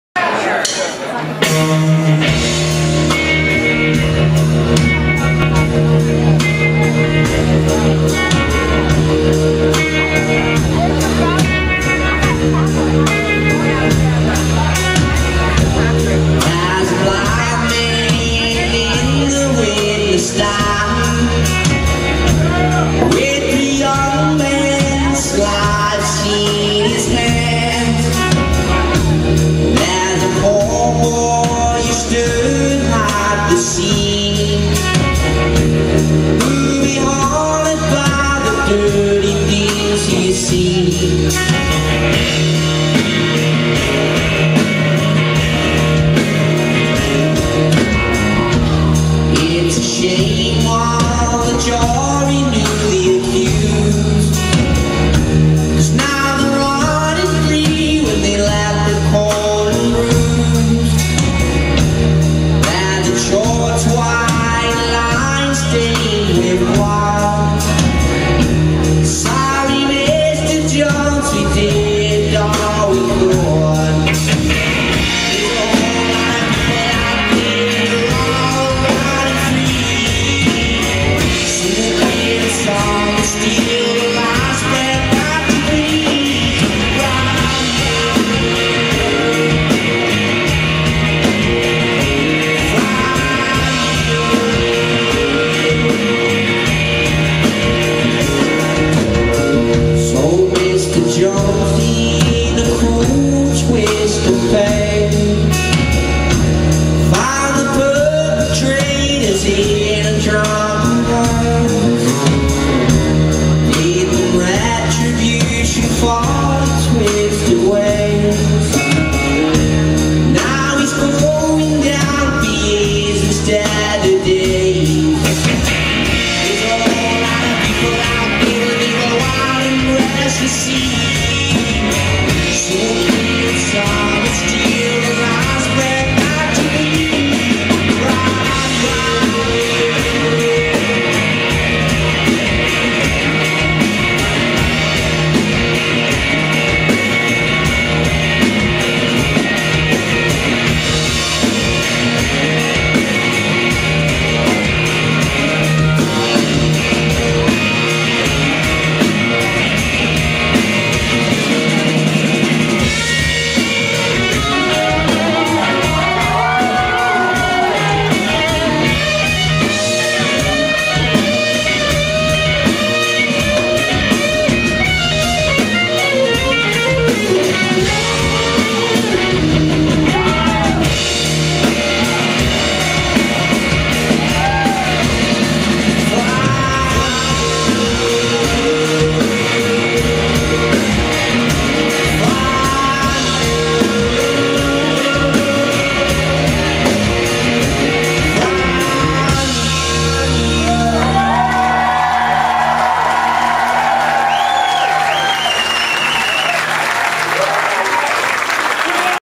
Jake Bugg in Dallas, Texas
Recorded live in October of 2013 at the Kessler Theater in X+/North Oak Cliff